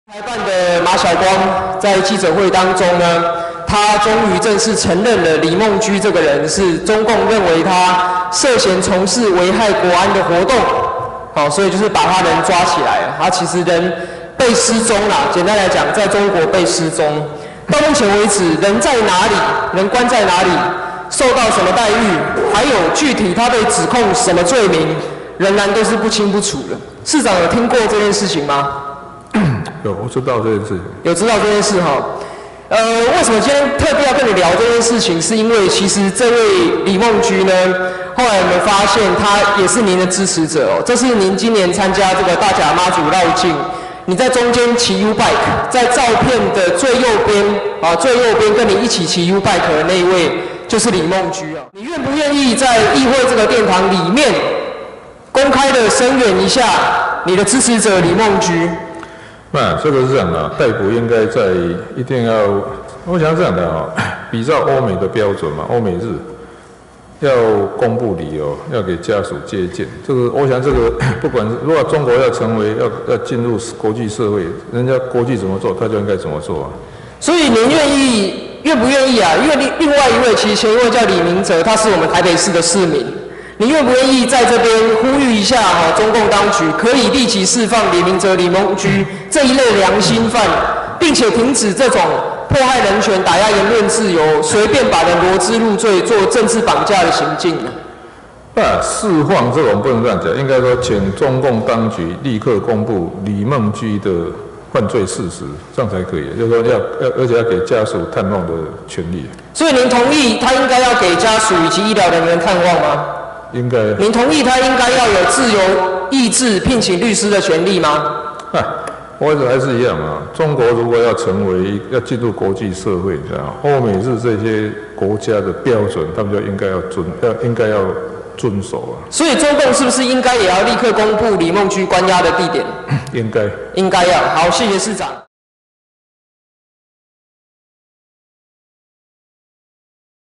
議員要求聲援李孟居 柯文哲：中共當局應立刻公布犯罪
枋寮鄉政顧問李孟居赴港旅遊赴中經商失聯至今日已23天，中國官方今坦承捉走李孟居，台北市議員苗博雅今在議會問到柯文哲此事，指李孟居是柯的支持者，今年柯文哲赴中南部時李孟居也在旁，柯是否願意公開聲援李孟居，呼籲中國不要破壞人權？柯文哲則說：「釋放我不能這樣講，但中共當局應立刻公布李孟居的犯罪事實」